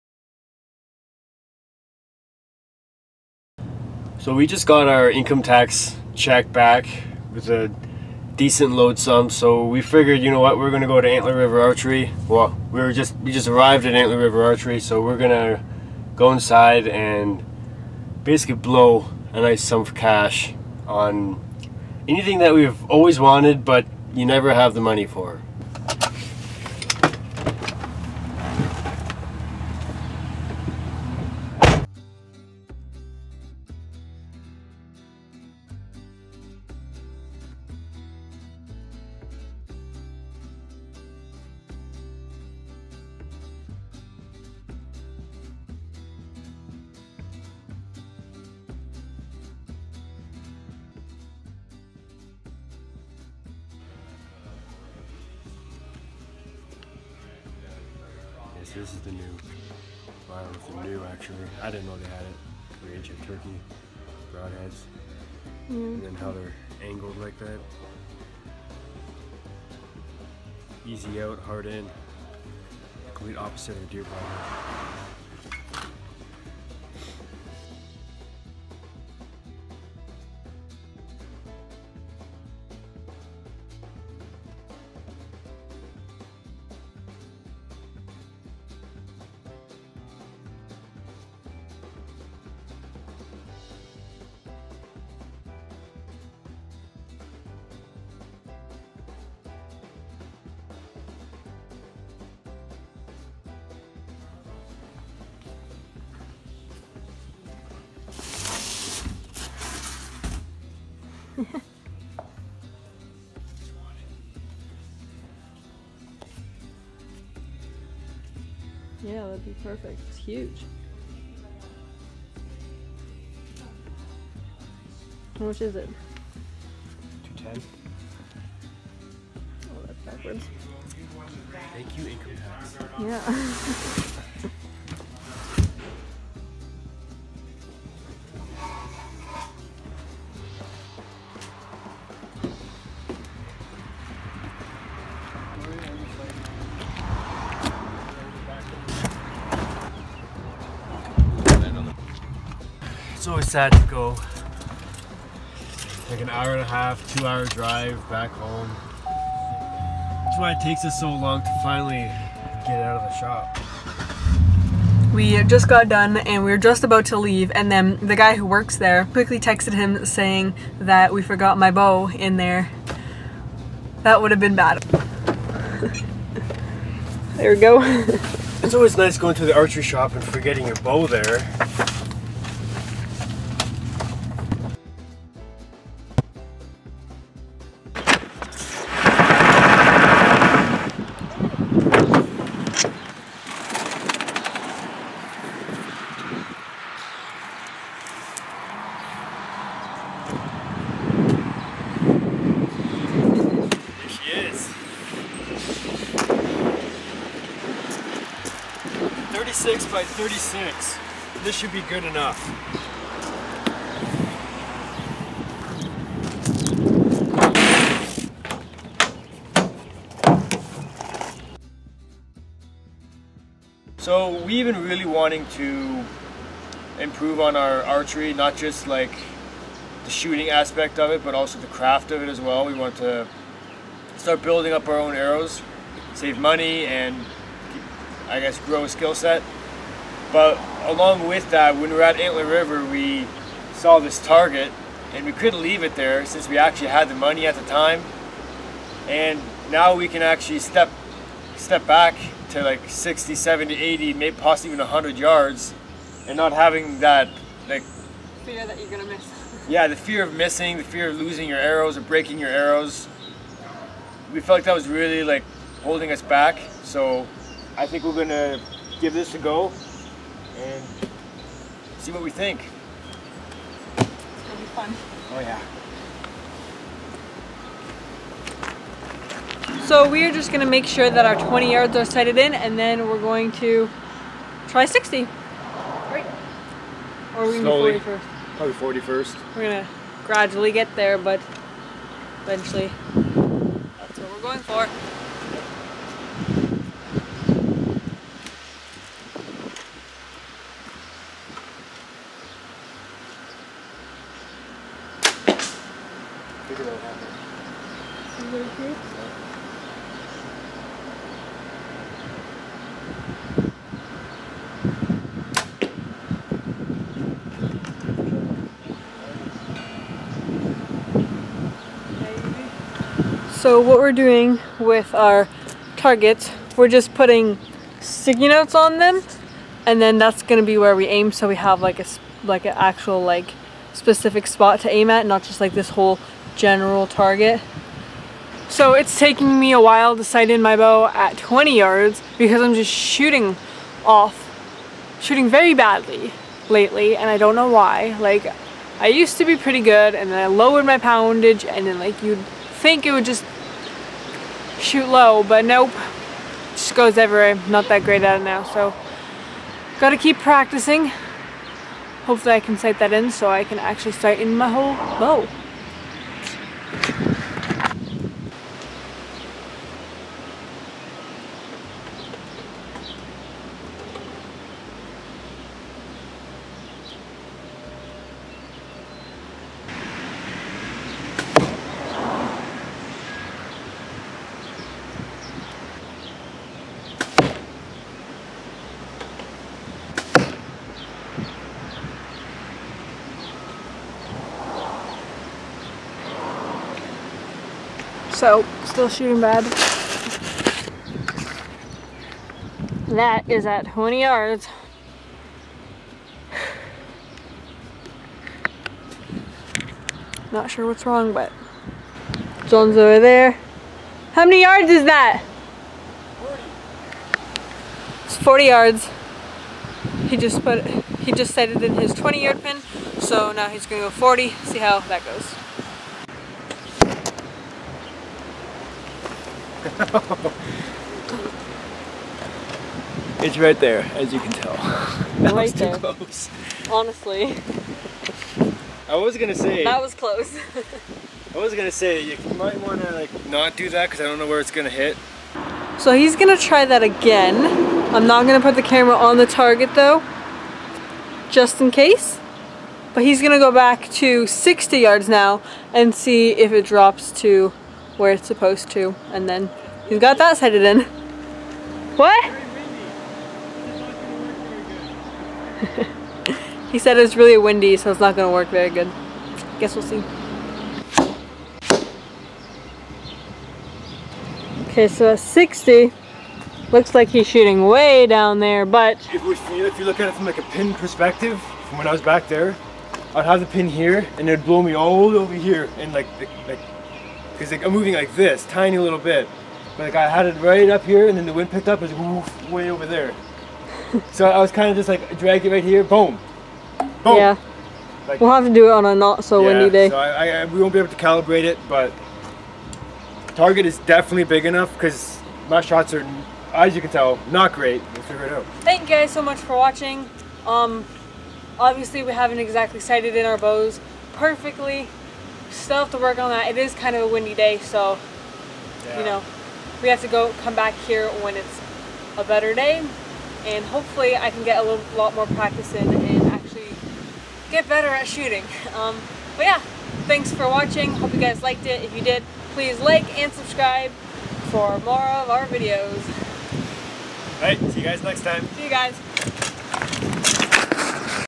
We bought a HUGE archery target! Sighting in our compound bows!
We bought a huge archery target from Antler River Archery! gotta love income tax money!!
We’re sighting in our compound bows to get ready for hunting season!

John: @johnredecop_
Jess: @jessicaredecop22